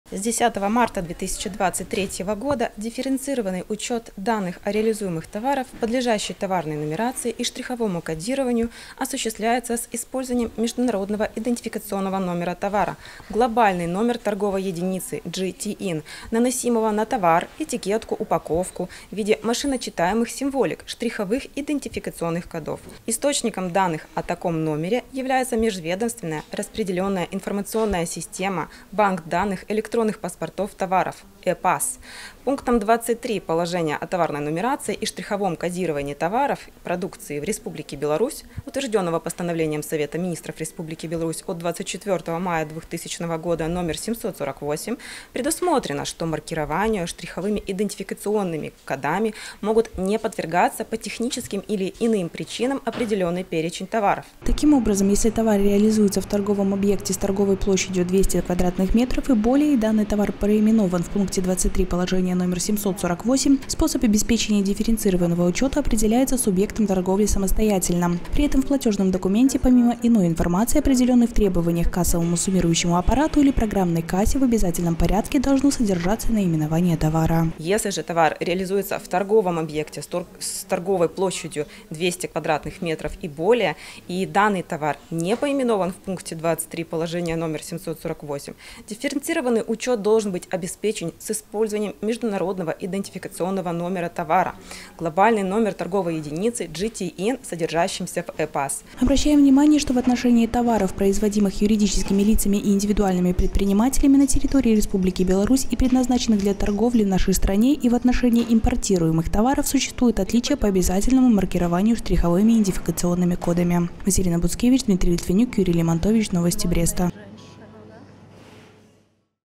2024-04-09 Налоговые новости : дифференцированный учет
Дифференцированный учет - это автоматизированный учет, предусматривающий наличие у юридического лица и индивидуального предпринимателя информации о наименовании, количестве и стоимости предлагаемых к реализации товаров и реализуемых товарах в торговых объектах. В настоящее время субъекты хозяйствования, осуществляющие продажу товаров в торговом объекте с торговой площадью 200 квадратных метров и более, обязаны использовать кассовое оборудование, обеспечивающие дифференцированный учет данных о товарах, а также формирование в платежном документе наименования товара. Как избежать нарушений законодательства нашей съёмочной группе рассказала главный государственный налоговый инспектор отдела информационно-разъяснительной работы ИМНС по Ленинскому району г. Бреста Людмила Лескович.

*Наши контакты:*
✅Телефон горячей линии:
54-63-67